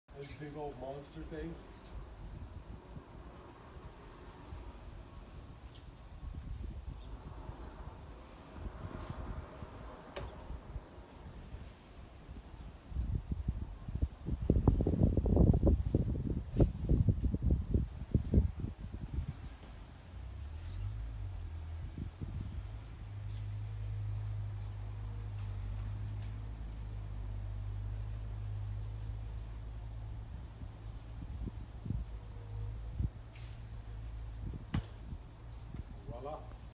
Custom Lift Slide door by Quantum Windows and Doors
Very large custom Sapele Mahogany Lift/slide doors - 93" x 142" (7'9"w x 11'10"h) panels, 181" x 142" (15'1" x 11'10") clear opening. All integrated into a custom Sapele 3 sided window wall. Designed by Walker Warner Architects.